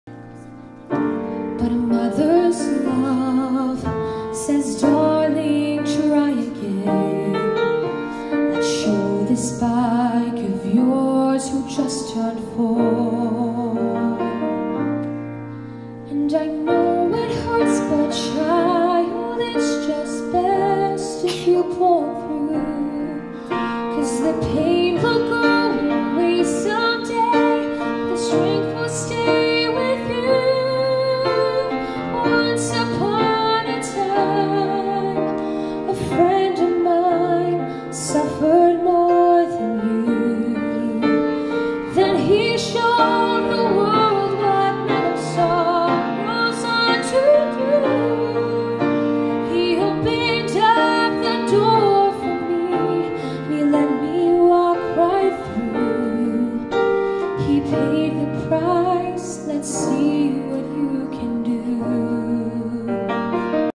Copy of Emily Henry 16 years old sings at Humanitarian Fireside.
Emily Henry, 16 vocalist. Masa Fukuda, pianist. Humanitarian Fireside, with Alan Osmond, Nathan Osmond, and Steve Kocherhans. Cans of food and coats were collected for the Road Home shelter in Salt Lake City. Emily had the privilege to sing.